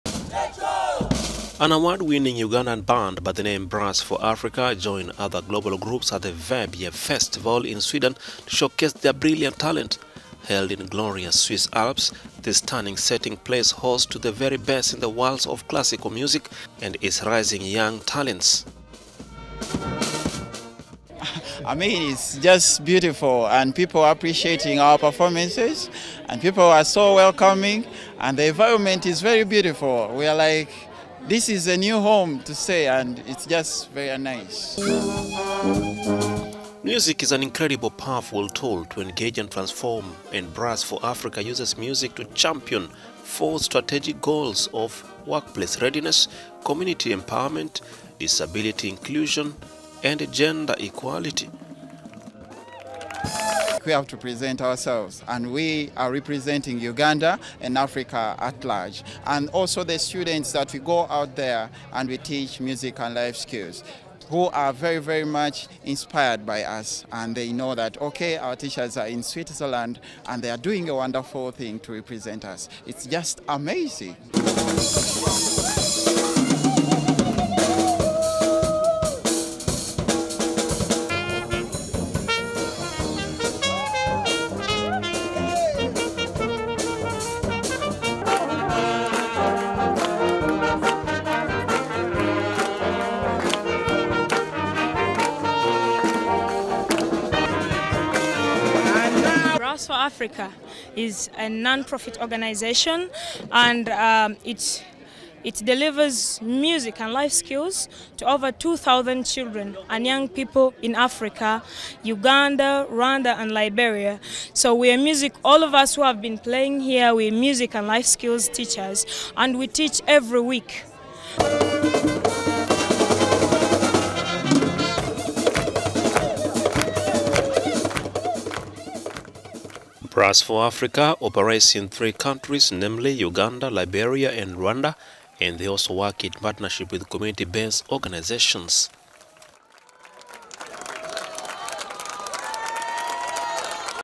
Award winning Ugandan band perfoms at classical music festival in Swiss
Brass for Africa joined other global groups at the Verbier Festival to showcase their brilliant talent. Held in the Swiss Alps, this stunning setting plays host to the very best in the world of classical music and its rising young talents.